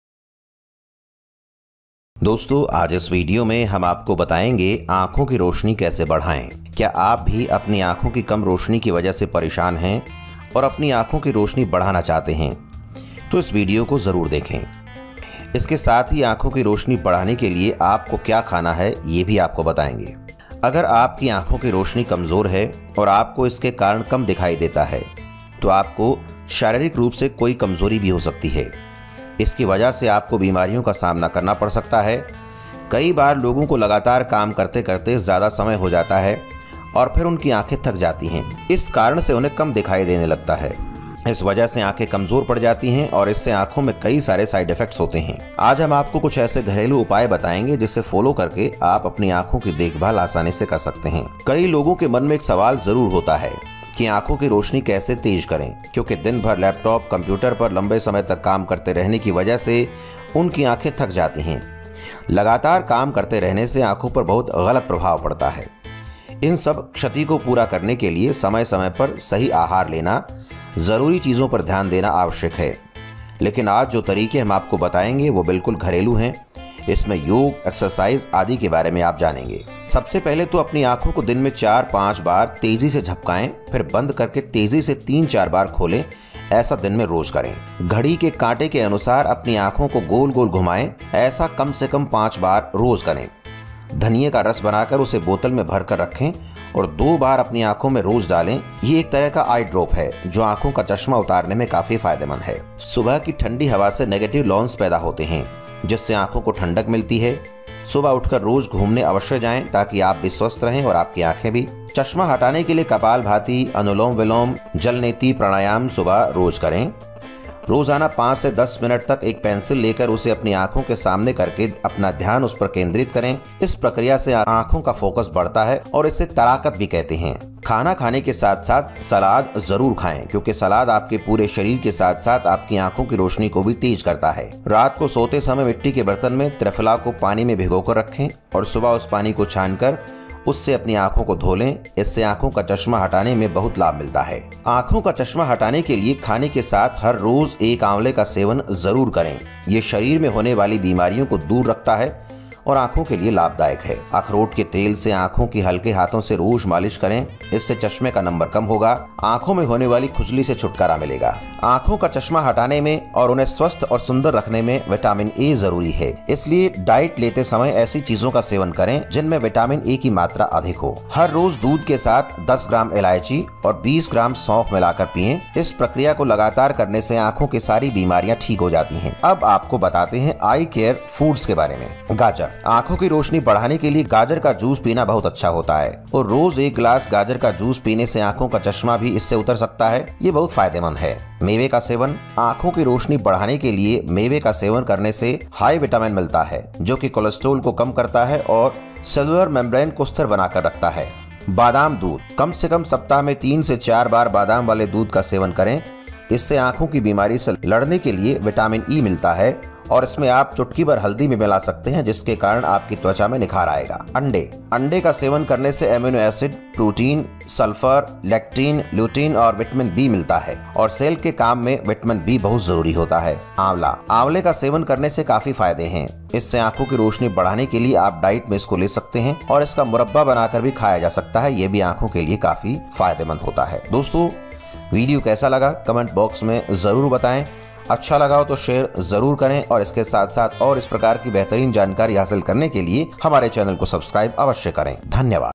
मैं अपनी आँखों से चश्मा कैसे हटाउ,बढ़िया उपाय कोनसे है, क्या खाऊं Home Remedy for Eye Sight
मैं आँखों से चश्मा कैसे हटाउ,बढ़िया उपाय कोनसे है, क्या खाऊं